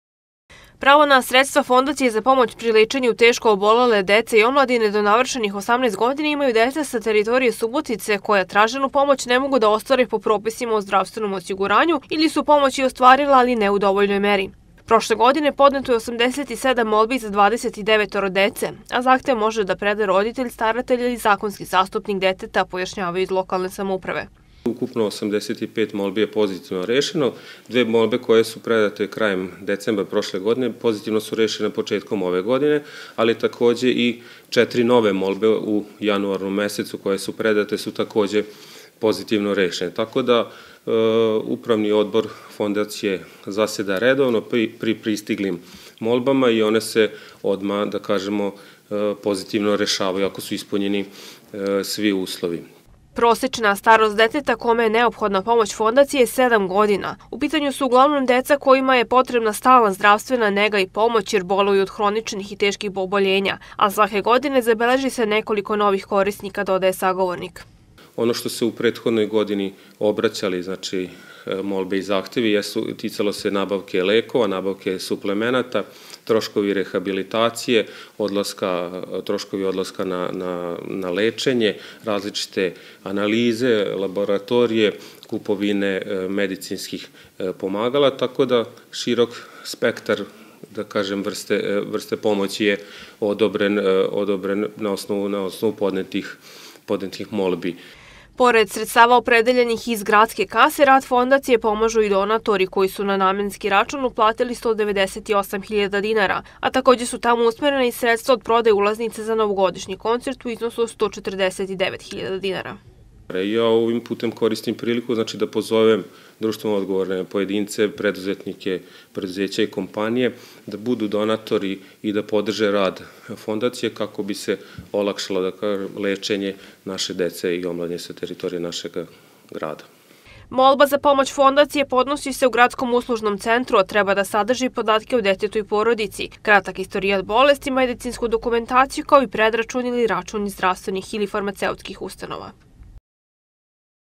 Od početka godine četiri nove molbe Fondaciji za lečenje teško obolele dece
Budžet Fondacije za pomoć pri lečenju teško obolele dece i omladine sa teritorije Grada Subotice za ovu godinu iznosu 4,5 miliona dinara,  od početka godine odobrene su četiri nove molbe, dok je u prethodnoj godini broj zahteva iznosio 87.

Pravo na sredstva Fondacije za pomoć pri lečenju teško obolele dece i omladine do navršenih 18 godina imaju deca sa teritorije Subotice, koja traženu pomoć ne mogu da ostvare po propisima o zdravstvenom osiguranju, ili su pomoć i ostvarila, ali ne u dovoljnoj meri. Prošle godine podneto je 87 molbi za 29 dece, a zahtev može da preda roditelj, staratelj ili zakonski zastupnik deteta, pojašnjavaju iz lokalne samouprave.

Prosečna starost deteta kome je neophodna pomoć Fondacije je sedam godina, u pitanju su uglavnom deca kojima je potrebna stalna zdravstvena nega i pomoć, jer boluju od hroničnih i teških oboljenja, a svake godine zabeleži se nekoliko novih korisnika, dodaje sagovornik.

Pored sredstava opredeljenih iz gradske kase, rad Fondacije pomažu i donatori, koji su na namenski račun uplatili 198.000 dinara, a takođe su tamo usmerena i sredstva od prodaje ulaznica za novogodišnji koncert u iznosu od 149.000 dinara.

Molba za pomoć Fondacije podnosi se u Gradskom uslužnom centru, a treba da sadrži podatke o detetu i porodici, kratak istorijat bolesti, medicinsku dokumentaciju, kao i predračun ili račun iz zdravstvenih ili farmaceutskih ustanova.